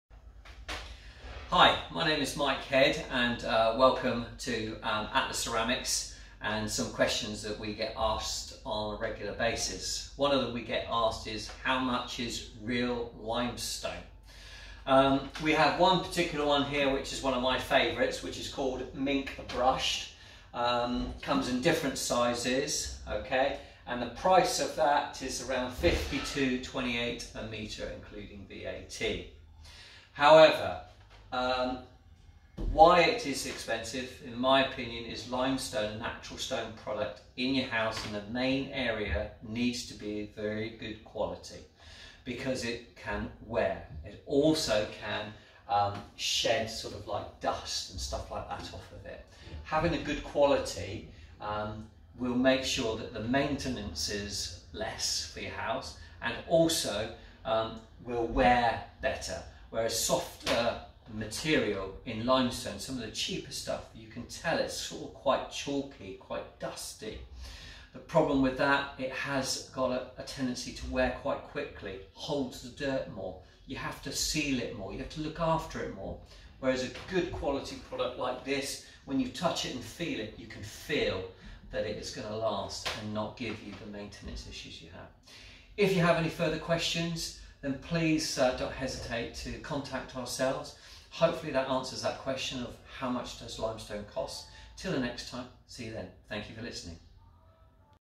How Much Do Limestone Tiles Cost?
How Much Do Limestone Tiles Cost? Find out how much your new tiling project will cost using limestone floor tiles compared to ceramic or porcelain.

View our range of limestone tiles here -